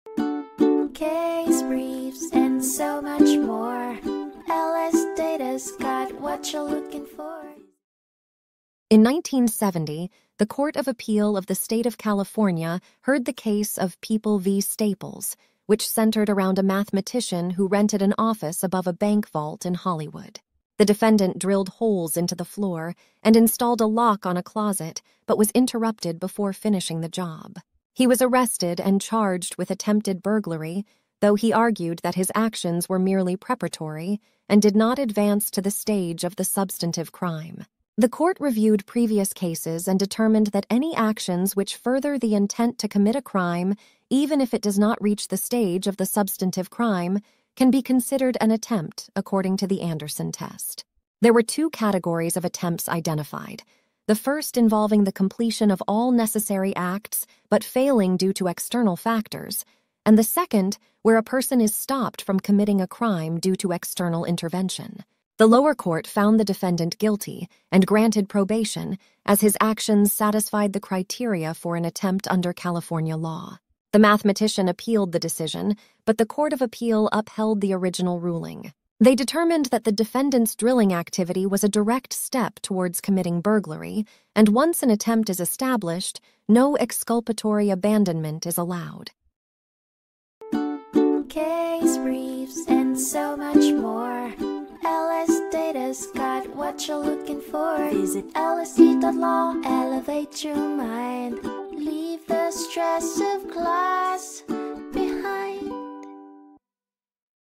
People v. Staples (1970) Overview | LSData Case Brief Video Summary
A mathematician drilled holes into his rented office's floor above a bank vault and installed a lock on a closet. He admitted to it but said it wasn't burglary because he didn't finish the job. The prosecution argued that he intended to commit burglary and did enough to qualify as an attempt. There is some confusion in California law about the definition of an attempt. The court reviewed previous cases and identified two categories of attempts. The defendant argued that he did not make an attempt because he was interrupted before he could finish the job. The relevant facts are what the defendant actually did to prepare for the burglary and whether that was sufficient to qualify as an attempt.

People v. Staples (1970)
Court of Appeal of the State of California
6 Cal. App. 3d 61